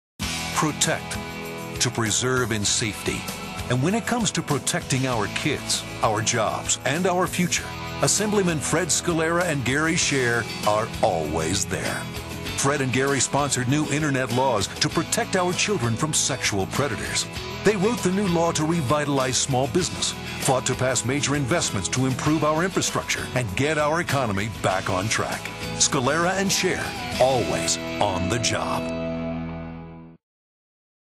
Fred Scalera & Gary Schaer - "Protect"
Fred Scalera & Gary Schaer for Assembly

When it comes to protecting our kids, our jobs, and our future, Assemblymen Fred Scalera and Gary Schaer are always there. Fred and Gary have sponsored new internet laws to protect our children from sexual predators. They wrote the new law to revitalize small business and ought to pass major investments to improve our infrastructure and get our economy back on track. Fred Scalera and Gary Schaer are always on the job.